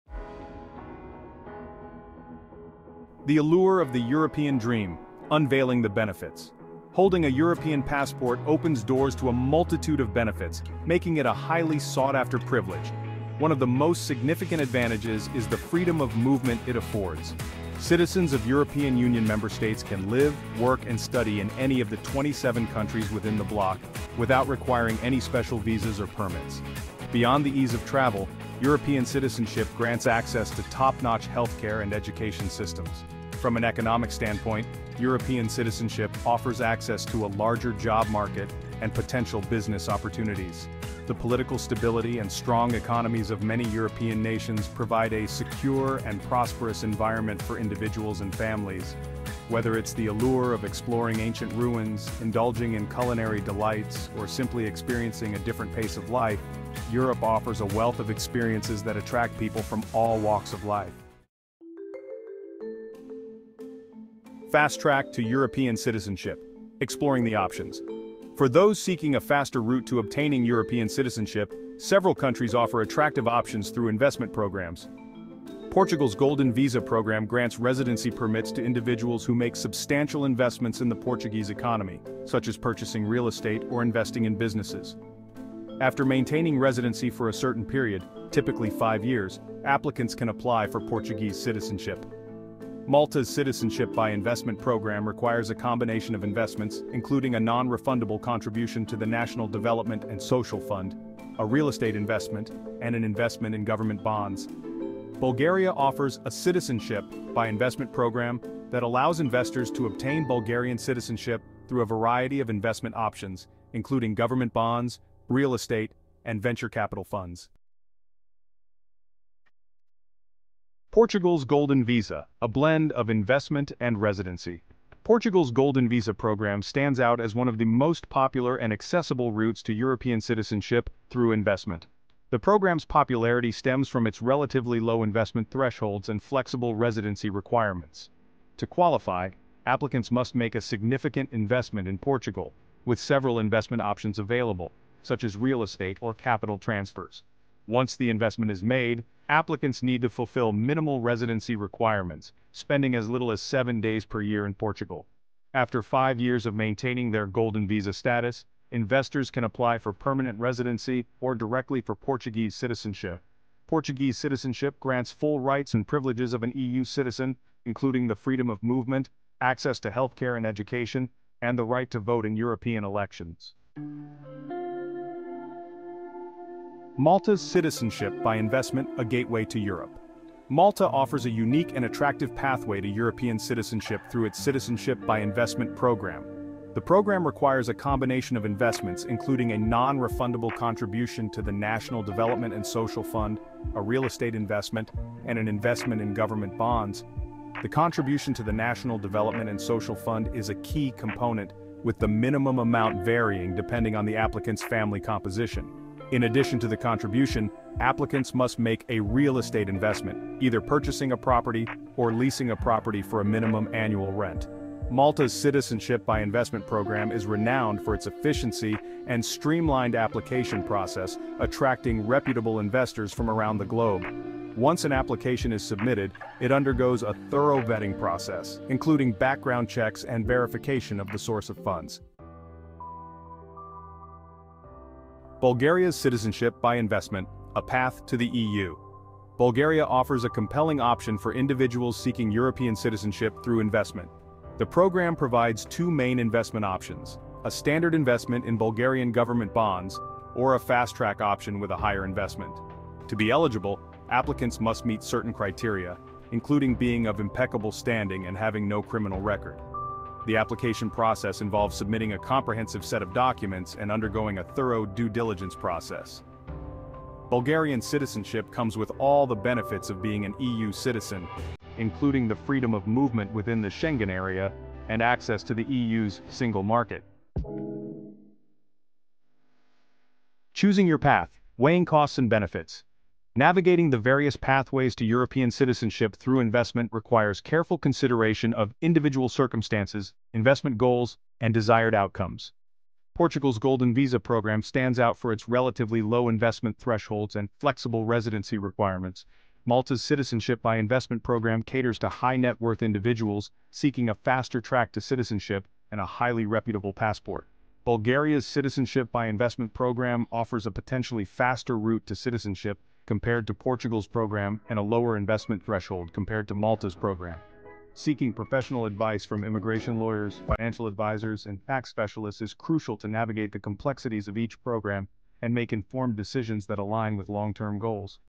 GET EUROPEAN CITIZENSHIP FAST AND AFFORDABLE IN 2024
Are you interested in obtaining European citizenship quickly and affordably in 2024? This video reveals the secrets to getting a European passport without breaking the bank. From residency requirements to investment opportunities, we'll explore the fastest and most affordable ways to become a European citizen. Whether you're looking for a second passport for travel, business, or personal reasons, this video will provide you with the information you need to make an informed decision. So, if you're ready to take the first step towards European citizenship, watch until the end and discover the opportunities available to you in 2024.